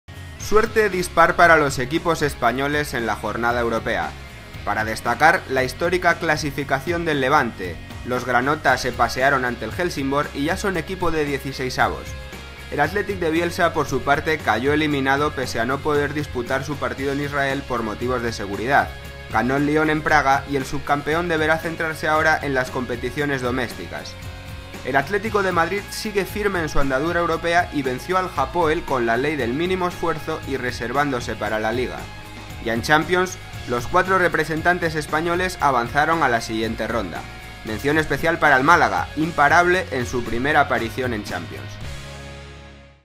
Suerte dispar para los equipos españoles en Champions y Europa League
Tras haberse disputados los partidos de Champions League y Europa League el Athletic Club de Bilbao queda fuera de la Europa League mientras el Atlético de Madrid y el Levante siguen luchando por el trono europeo. En la Champions los equipos de Primera División han tenido mejor suerte habiendo pasado todos a la siguiente ronda.